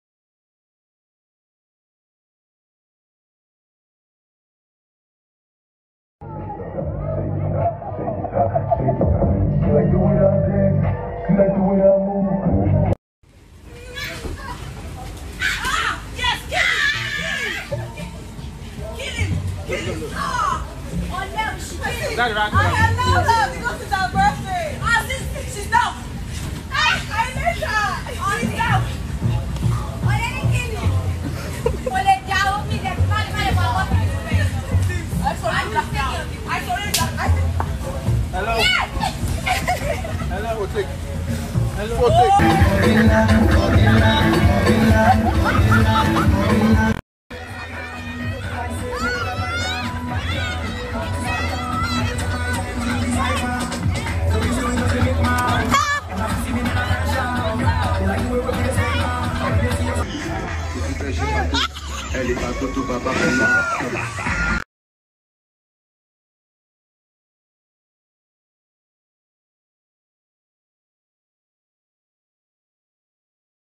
Videos and photos from Simi's 33 birthday party
Simi celebrate her 33 birthday with friends and family.